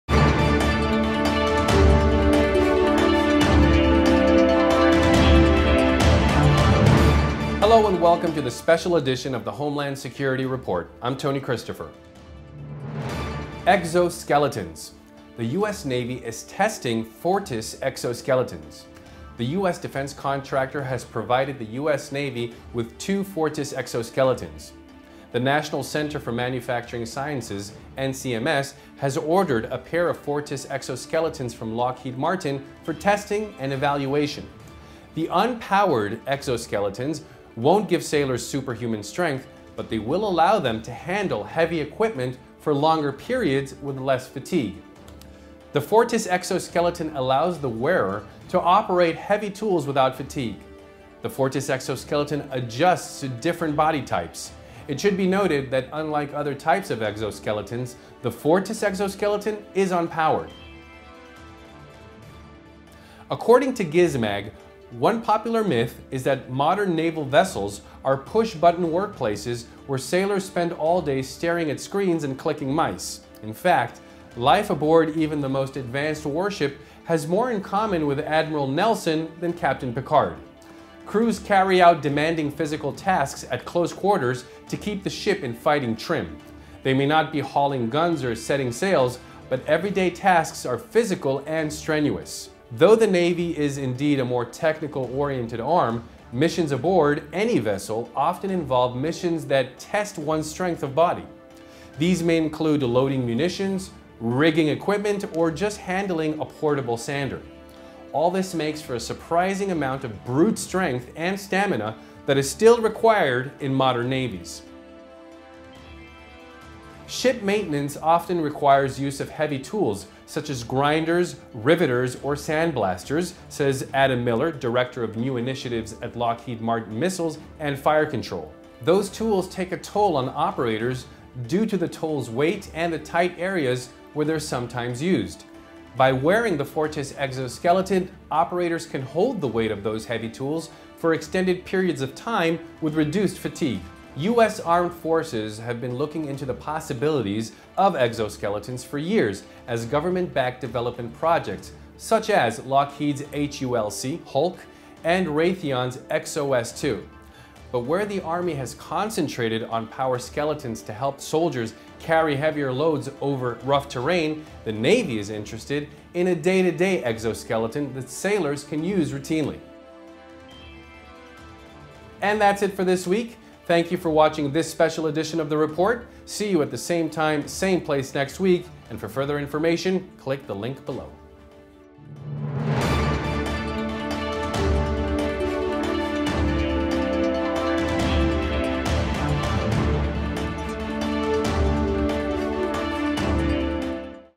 iHLS TV Special Video Edition: The US Navy is testing Fortis exoskeletons
iHLS TV Special Video Edition: The US Navy is testing Fortis exoskeletons: In this week's Special Video Edition, we discuss the US Navy and the US Army's use of exoskeletons.